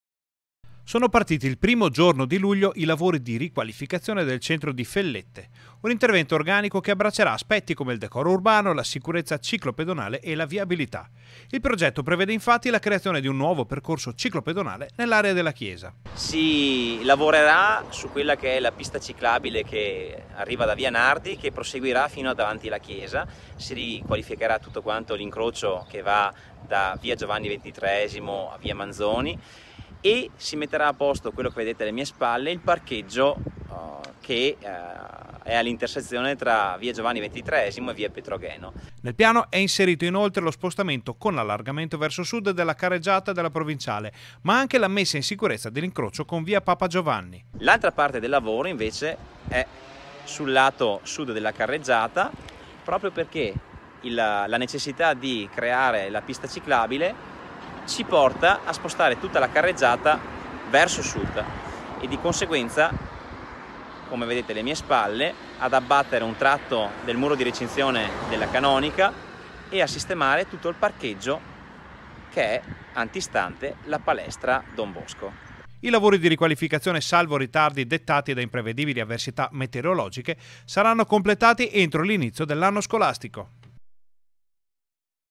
INIZIATI I LAVORI DI RIQUALIFICAZIONE DI FELLETTE CENTRO | 03/07/2020
TG Bassano del Grappa 03/07/2020 - ROMANO D'EZZELINO - Al via un importante lavoro di riqualificazione del centro della frazione di Fellette, a Romano d'Ezzelino || Sono partiti il primo giorno di luglio i lavori di riqualificazione del centro di Fellette. Un intervento organico che abbraccerà aspetti come il decoro urbano, la sicurezza ciclo-pedonale e la viabilità. Il progetto prevede infatti la creazione di un nuovo percorso ciclo-pedonale nell'area della chiesa - Intervistati SIMONE BONTORIN (Sindaco di Romano d'Ezzelino) - Servizio Ferdinando Garavello, immagini - Repertorio, montaggio Alessandro Costacurta - Segui Rete Veneta anche sul digitale terrestre! Visita il sito